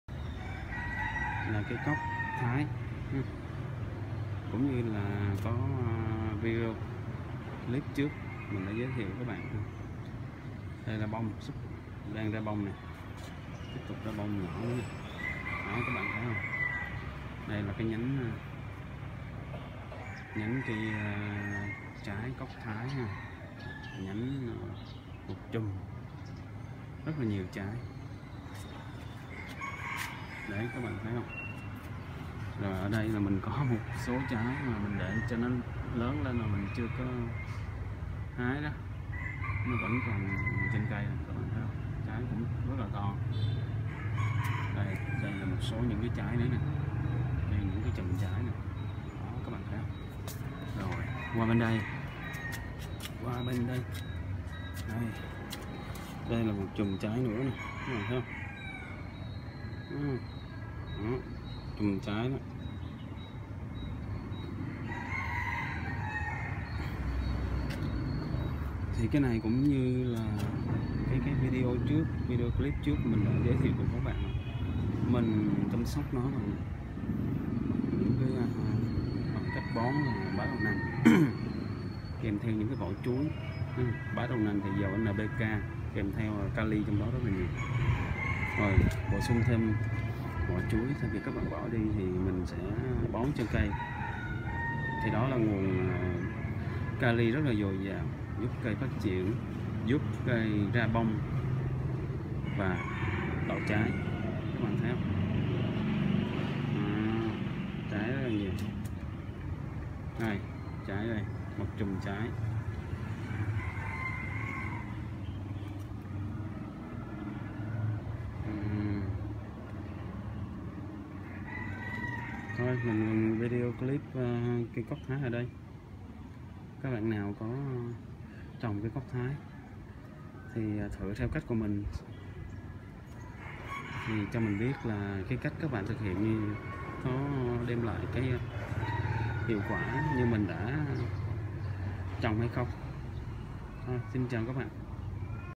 Chăm sóc cây cóc thái mau ra hoa đậu trái - thành quả
Sau một thời gian chăm sóc, cây cóc thái nhà mình ra hoa đậu trái rất nhiều và tiếp tục ra hoa ...